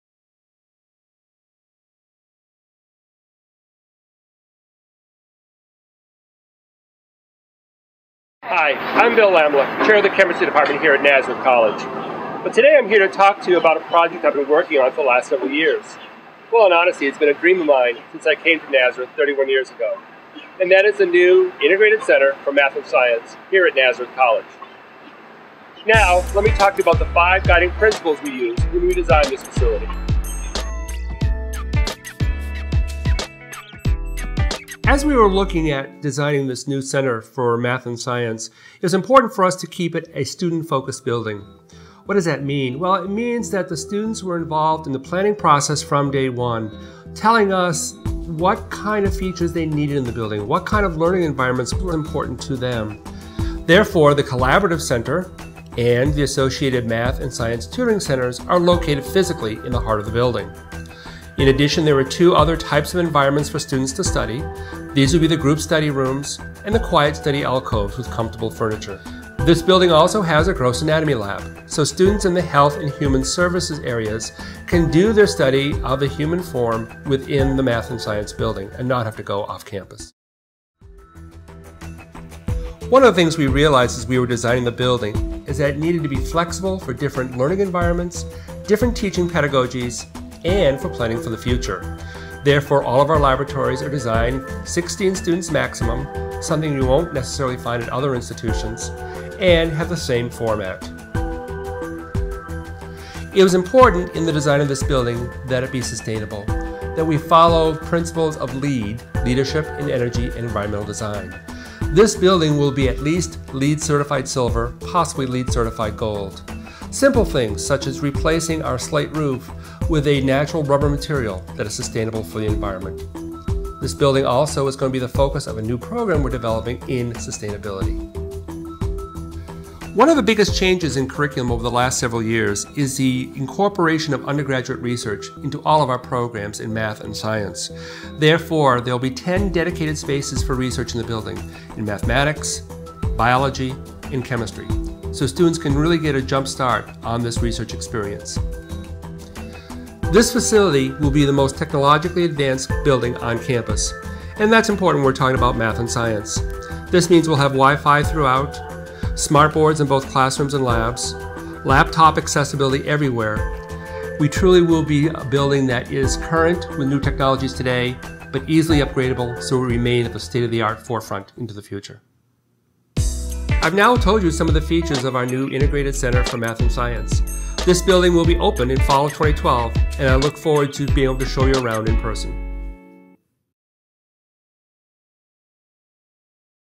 A Look at the Integrated Center for Math and Science
A look at Nazareth College's Integrated Center for Math and Science that is opening in the Fall of 2012. It is now under construction, and Dr. Bill Lammela, chair of the chemistry department at Nazareth guides you through five important principles about the building.